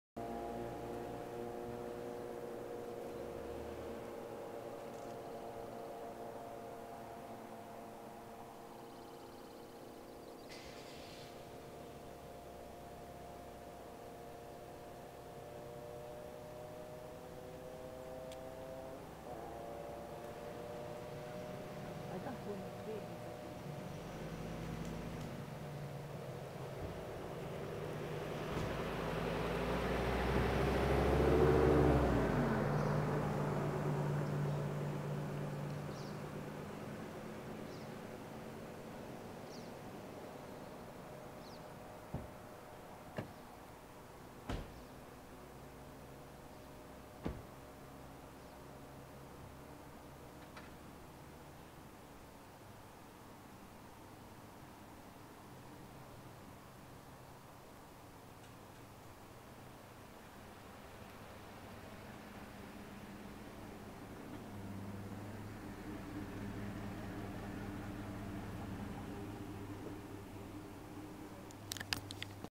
20190603 熊遭遇
親子３頭いる様子